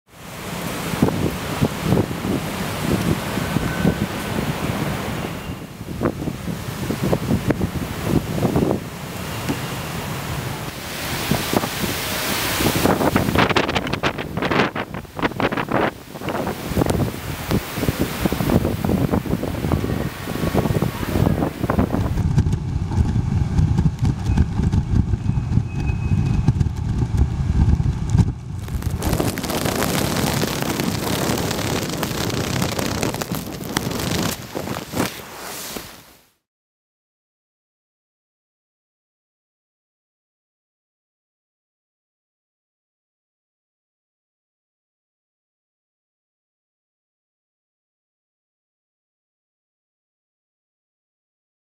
台風14号が九州南部を暴風域に巻き込み北上中
大型で非常に強い台風14号は9月18日、九州 南部を暴風域に巻き込みながら北上を続けている。